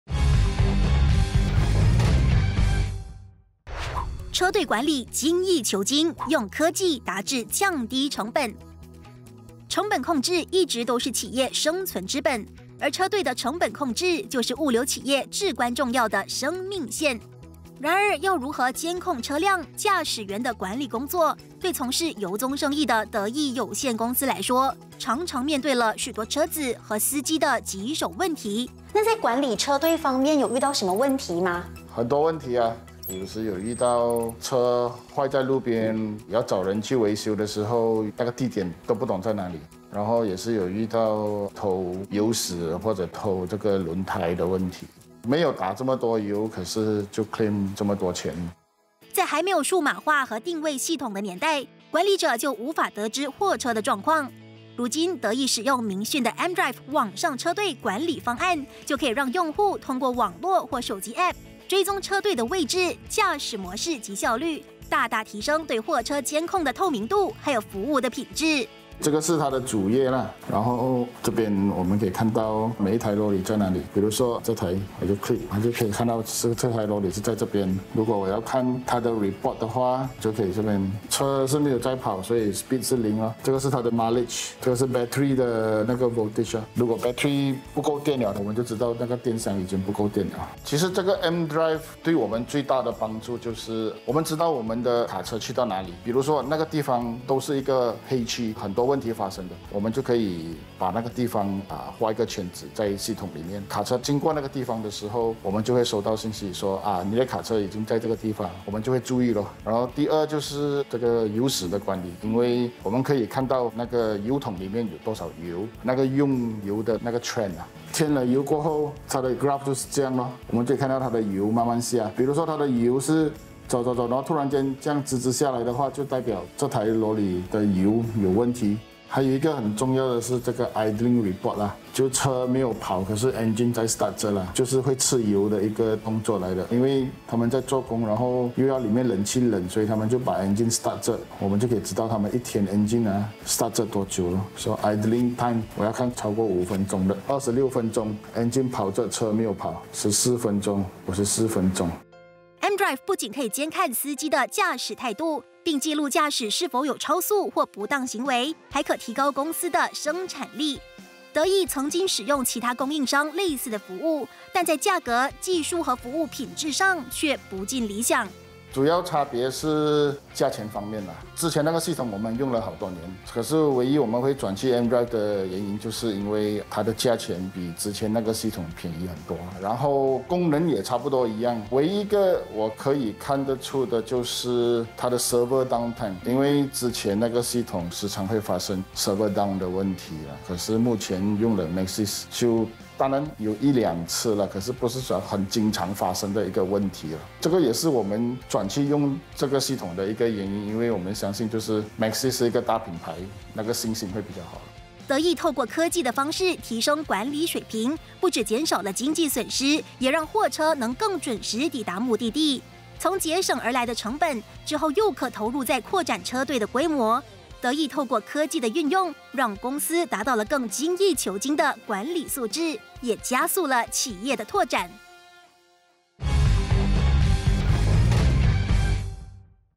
【企业大联盟 4.0】车队管理精益求精 善用科技控制成本
司机驾使态度不佳又常超速？看看运输业老板出招！